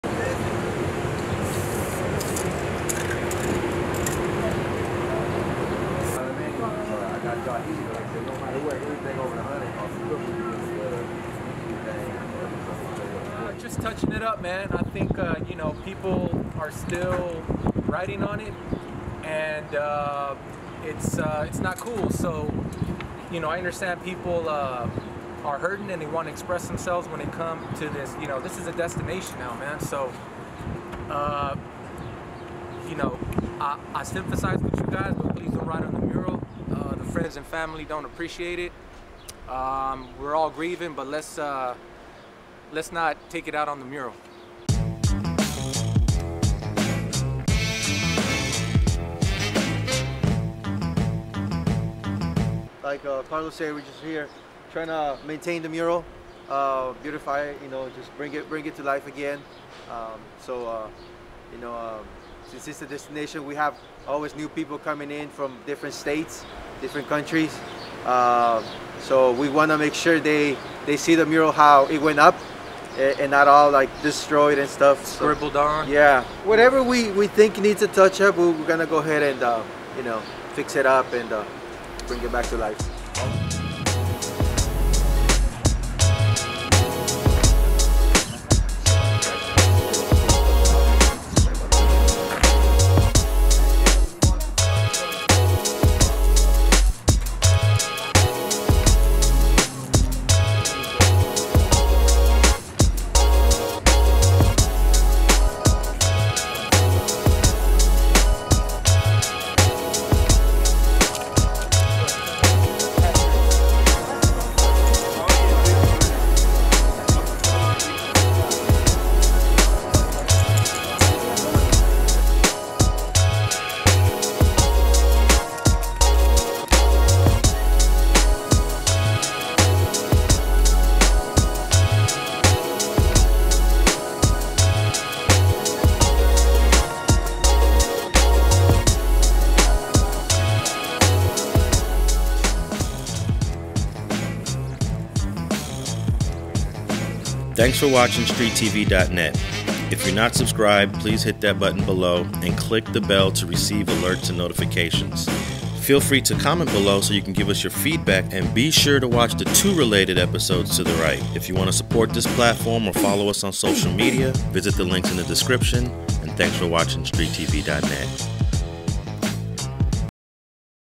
Artists touch-up Nip$ey Hus$le Mural after it was defaced
Last week, the mural on Slauson just off Crenshaw in the alley west of the Shell Gas Station was defaced in the form of unwanted graffiti written over it. On Sunday June 28, 2020, the original artists Danny Mateo @DannyMateo and Carlos Gomez @cgomez220 came out to the mural and touched it up and brought back the Nip$ey Hus$le mural to its original grandeur.

► Web site article: Coming Soon..